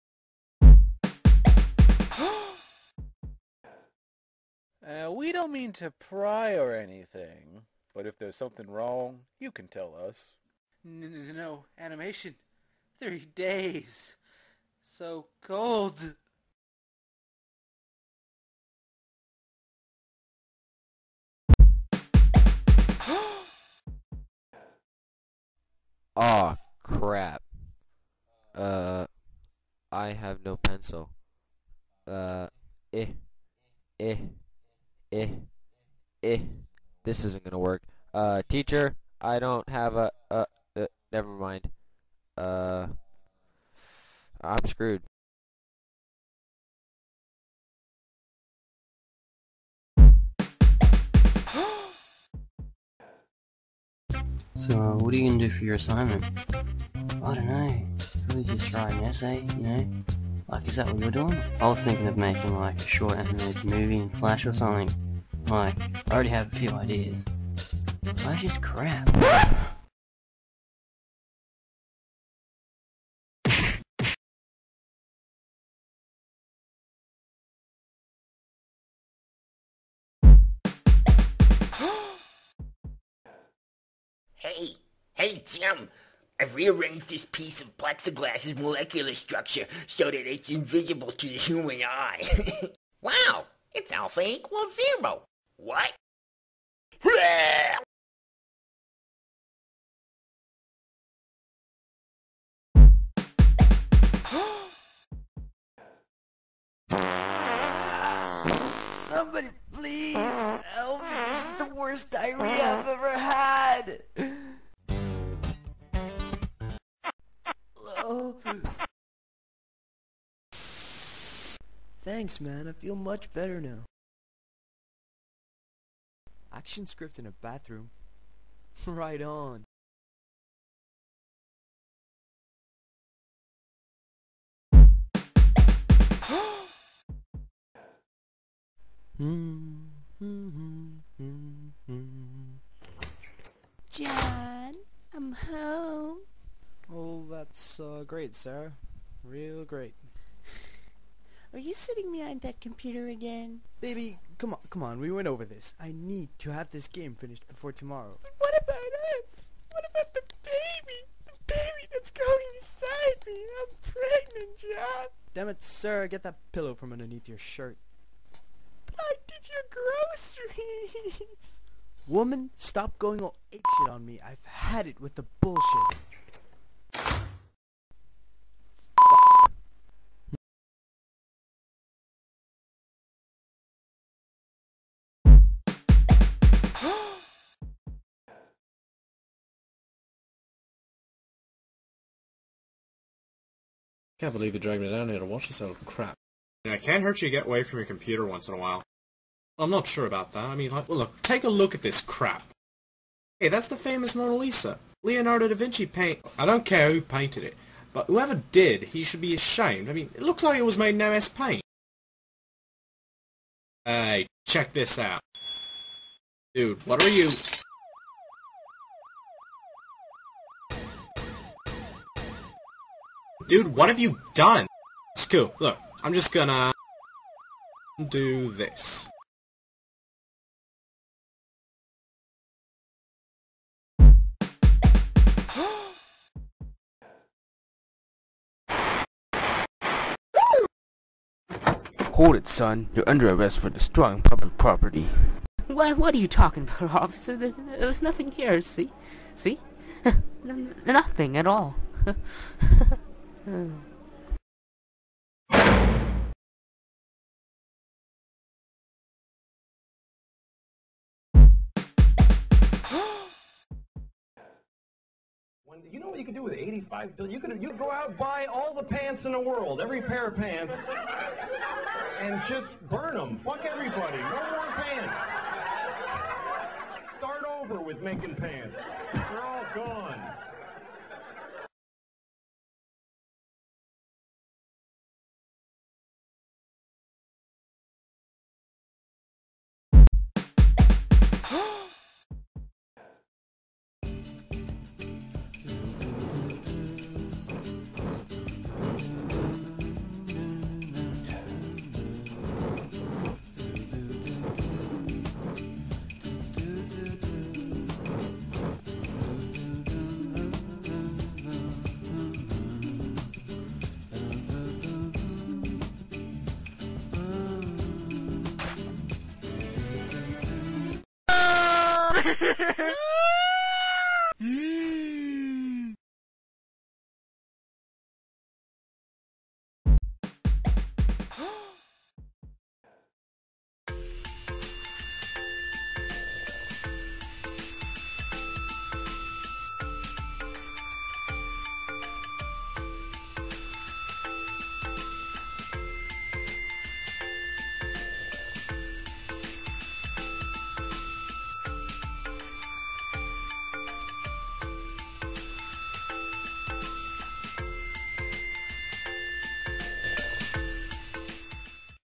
10 Signs You Make Too Much Flash
An animation collab I organized ages ago on Newgrounds. For full credits check it out on there